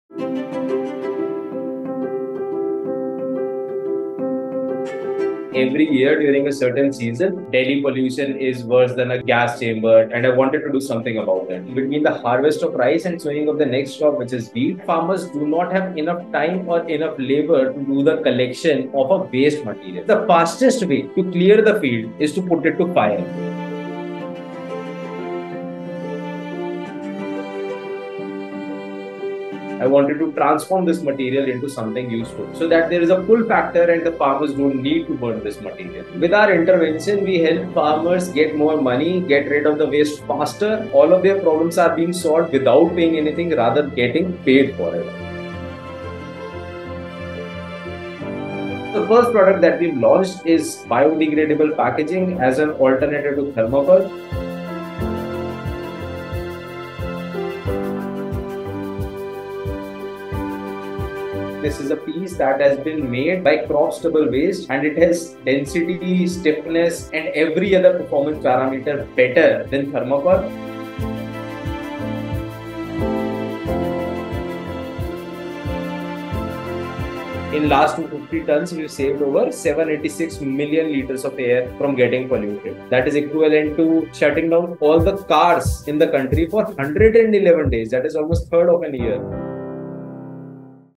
Faridabad-based startup Dharaksha is converting wasted rice straw stubble to packaging material
Every year, the smoky haze and severely polluted AQIs in Delhi and sorrounding areas make headlines. Stubble burning majorly contributes to the problem. Dharaksha converts paddy straw stubble into eco-friendly packaging material. Founded by Arpit Dhupar and Anand Bodh in 2019, this startup is on a mission to make Delhi's air cleaner by reducing stubble burning through sustainable solutions and keeping thermocol away from our land-fills.